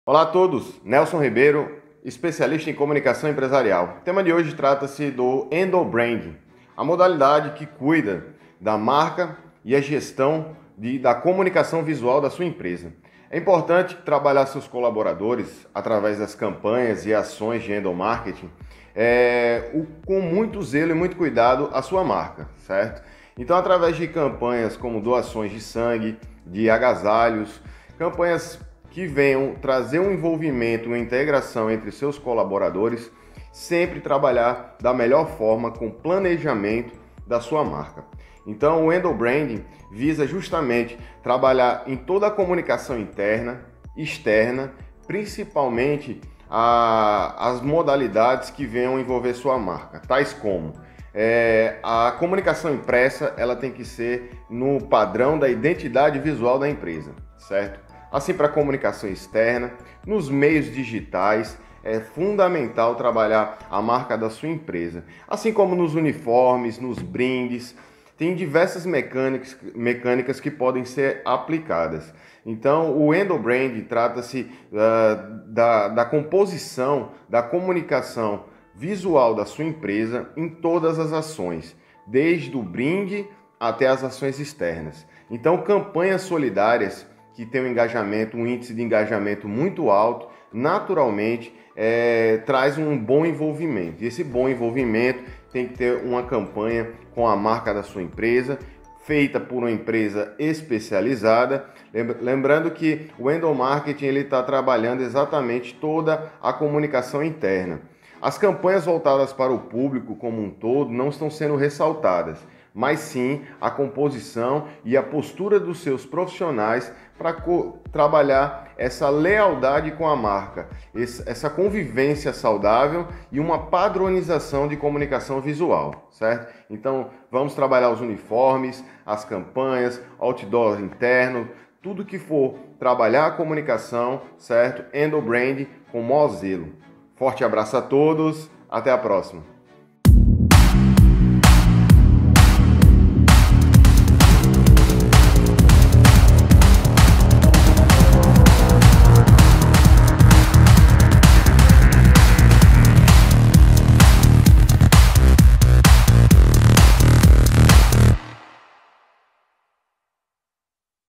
ENDOBRANDING NO
A gestão da marca e o planejamento da Comunicação Visual nas campanhas e ações de Endomarketing são fundamentais para um padrão de comunicação interna .

LiveEndomarketing é com a @status.empresarial !!! 🎯 empreenda com excelência em gestão de equipes, potencializando & motivando seus talentos.

Dicas sobre boas práticas de Endomarketing com o Especialista em Comunicação Empresarial @nelsonribeiro.oficial .